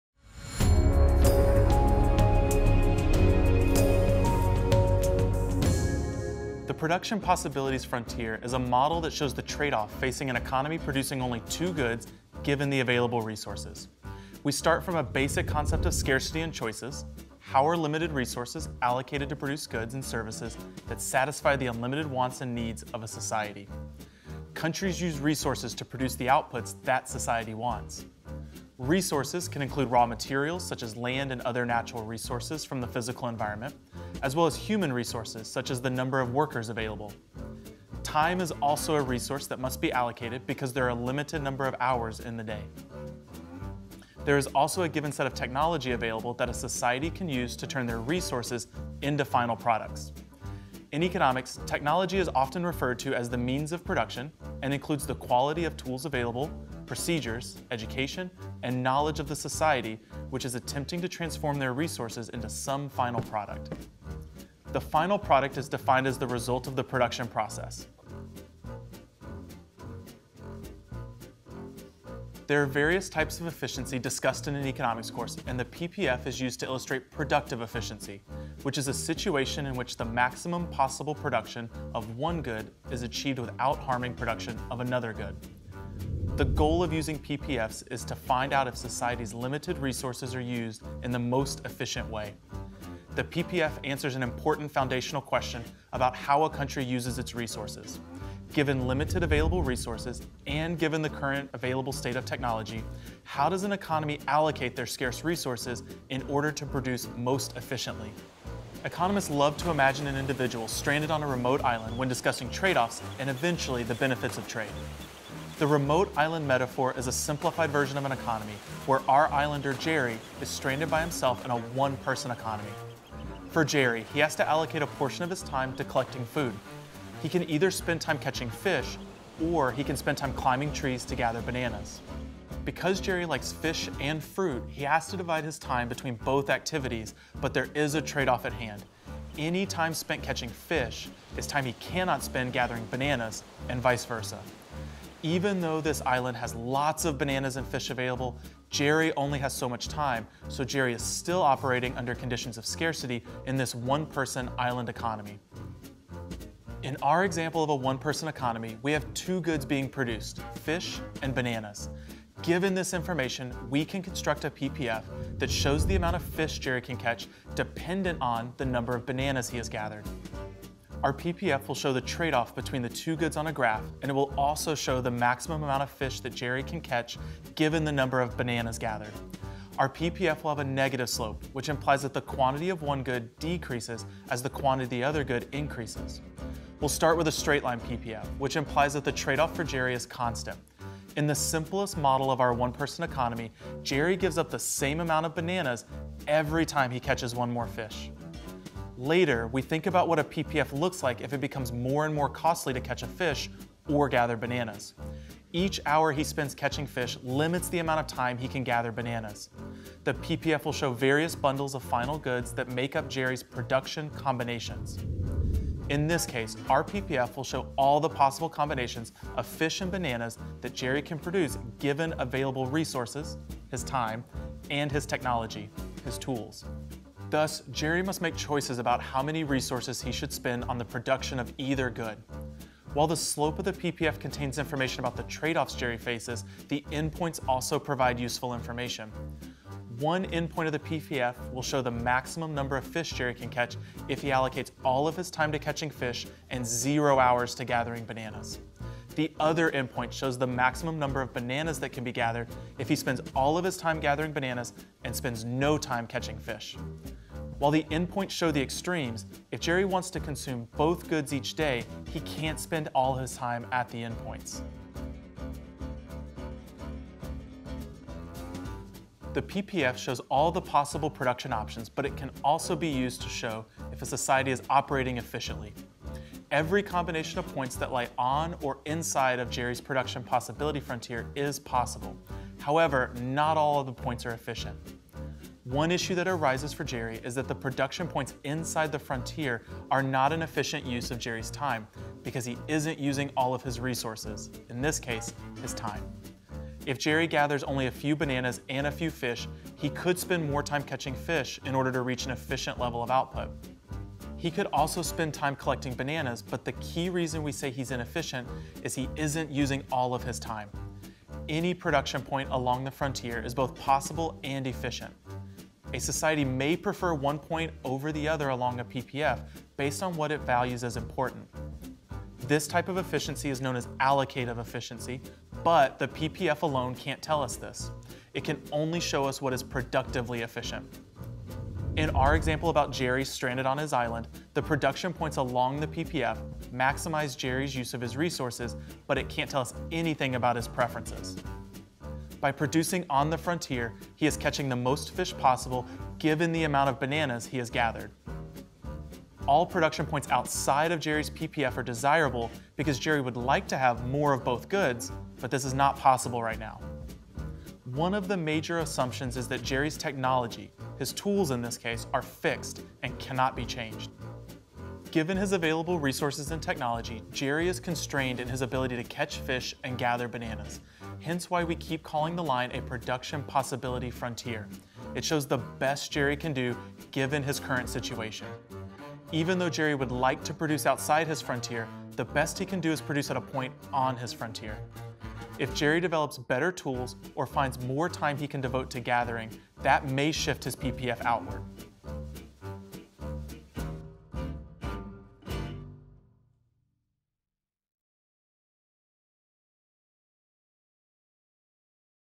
Production Possibilities Frontier | Microeconomics
Summarize videos instantly with our Course Assistant plugin, and enjoy AI-generated quizzes Learn all about production possibilities frontier in just a few minutes! Professor Jadrian Wooten of Penn State University explains modeling a production possibilities frontier to show the tradeoff of production between two goods.